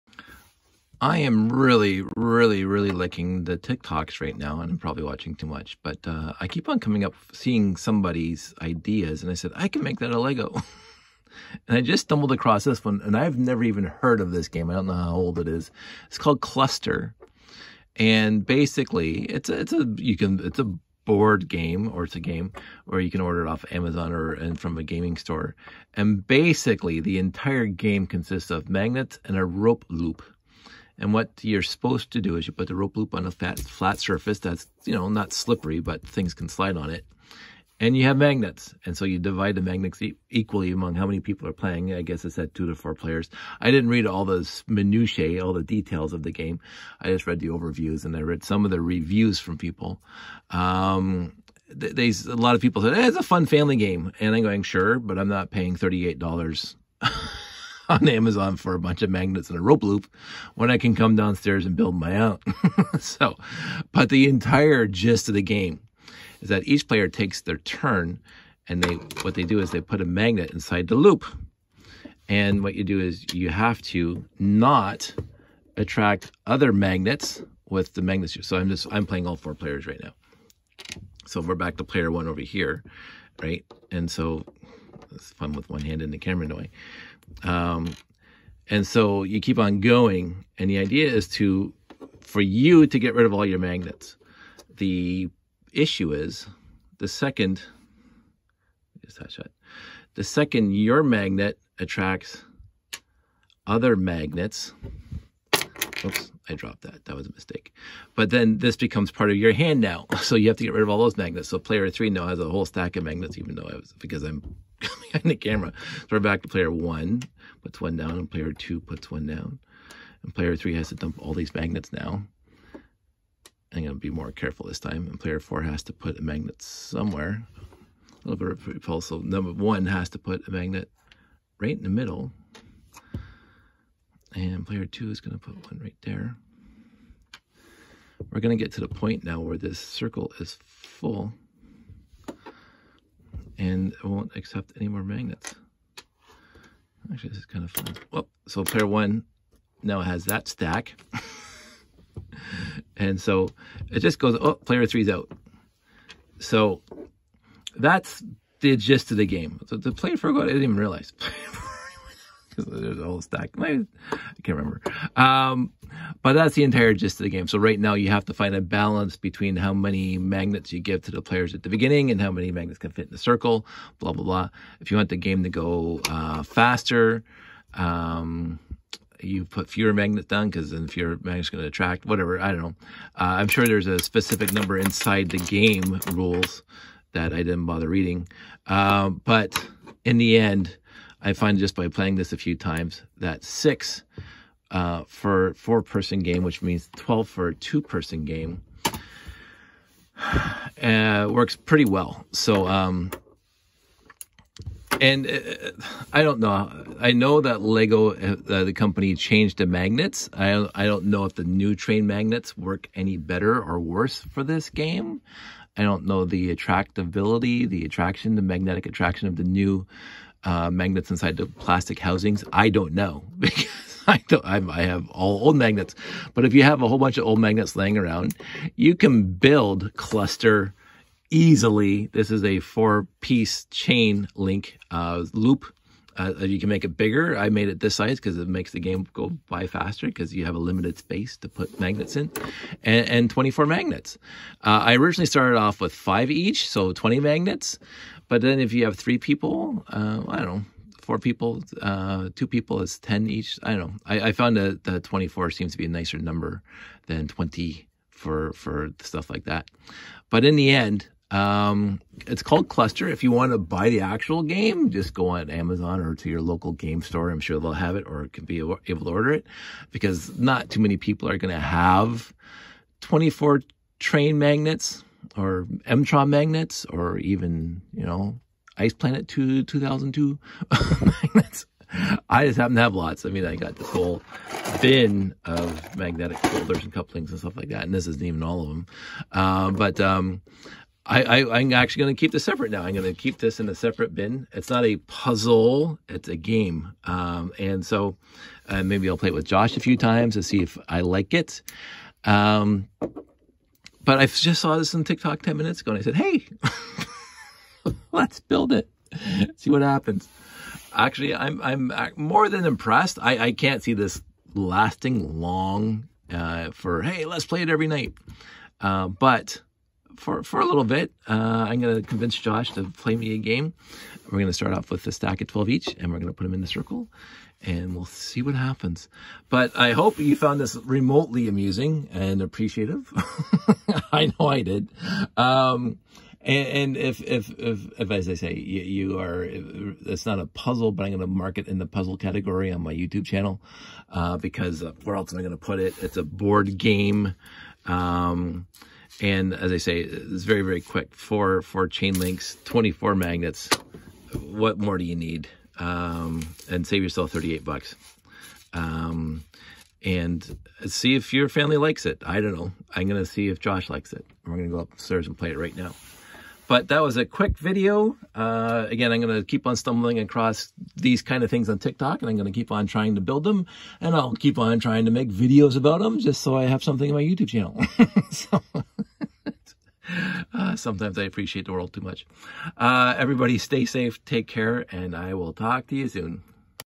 DKP - LEGO-ized Kluster Game!
Saw the Kluster game on TikTok and I said ‘I can make this out of lego!’
Here’s a ‘brief’ intro to the game.